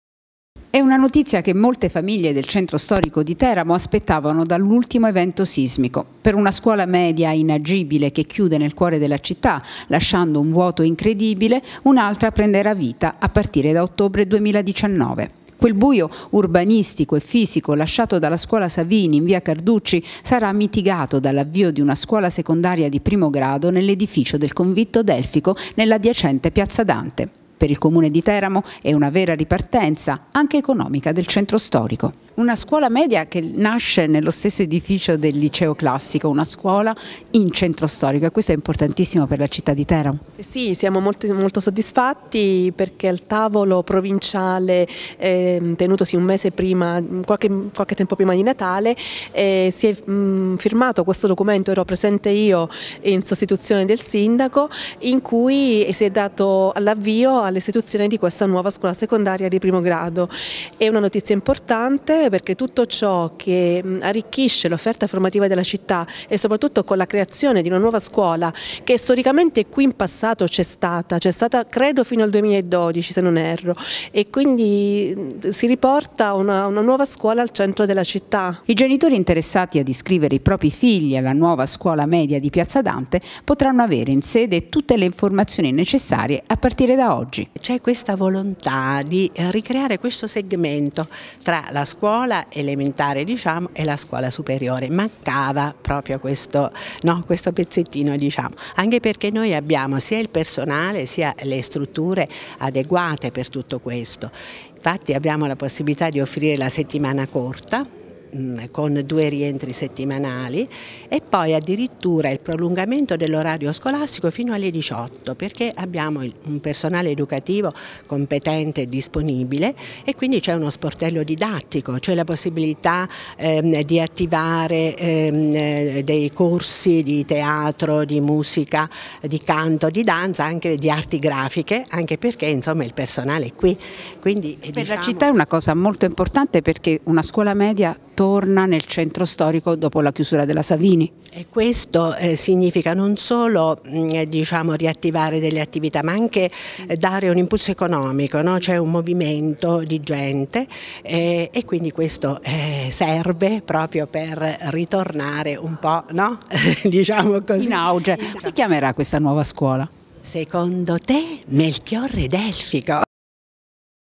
Una nuova scuola media in centro riporta fiducia in città
A più di due anni dall'ultimo sisma che ha reso inagibili decine di edifici scolastici, il centro storico di Teramo sembra riprendere vita con l'apertura di una nuova scuola media. La sede sarà quella del Convitto Melchiorre Delfico di Piazza Dante e sono aperte le iscrizioni